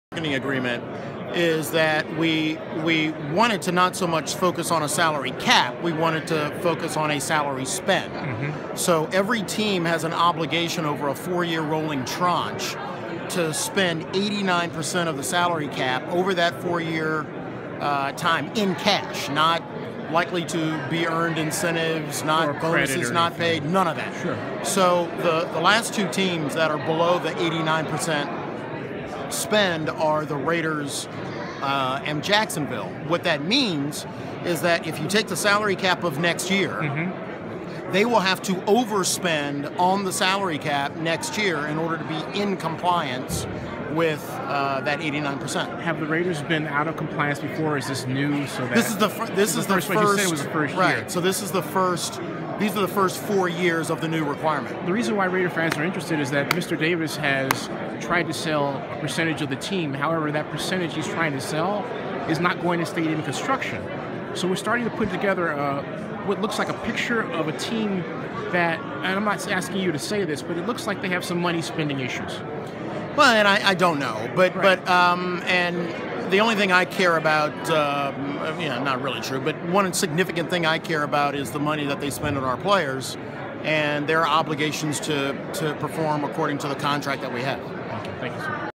Demaurice Smith Says Oakland Raiders Underpaid NFL Cap In 2015
Zennie62 According to NFLPA Executive Director Demaurice Smith, the Oakland Raiders are one of two NFL teams that are under the 89 percent cash spending requirement for the NFL Cap. This means the Raiders will have to spend that amount they are short by during the very next fiscal year in order to make up the difference.

The question is why are the Raiders spending less cash on their players than the other NFL teams?  The other organization with the problem is the Jacksonville Jaguars.

Is this lack of money?  Bad money management?  Both?

- vlog produced by Zennie Abraham Zennie62 Blog - youtube video - Oakland Raiders highlights and commentary